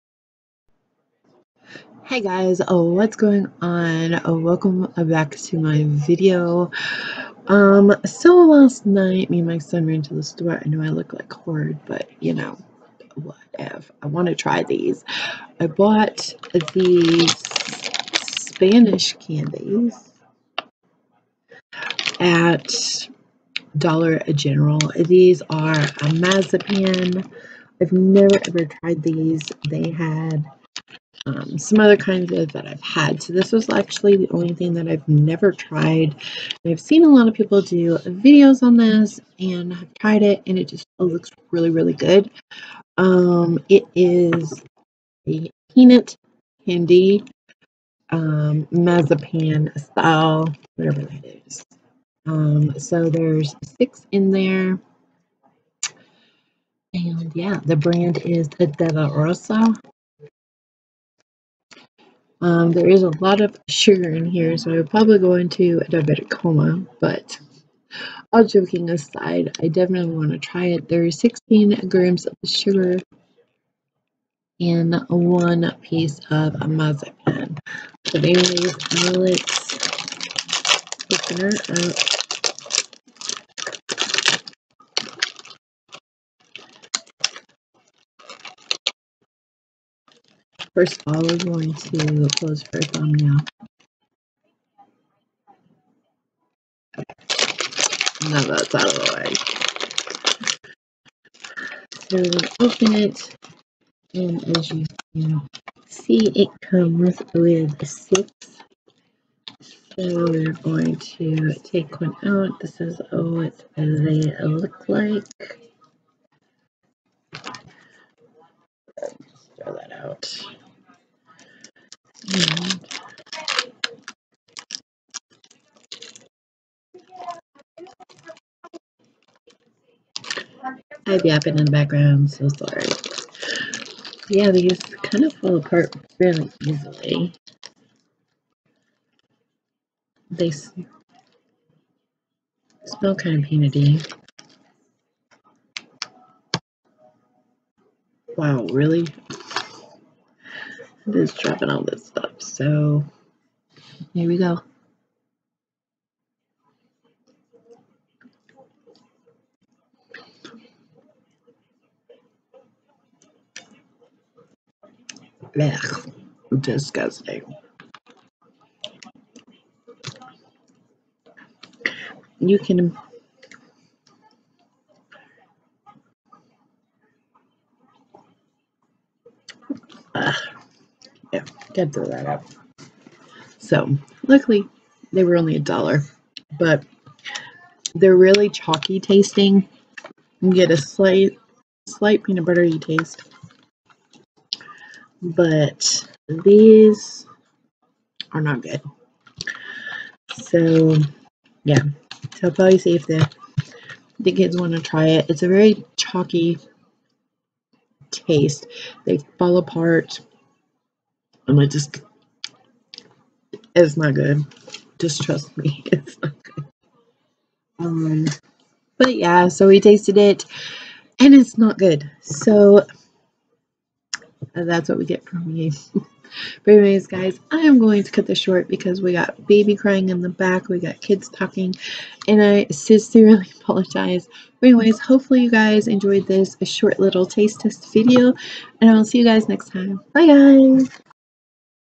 Spanish candy Taste Test...FAIL😱😱😱
okay guys video a little busy cause of the kids and baby but did a short taste test video hope you enjoy my reactions lol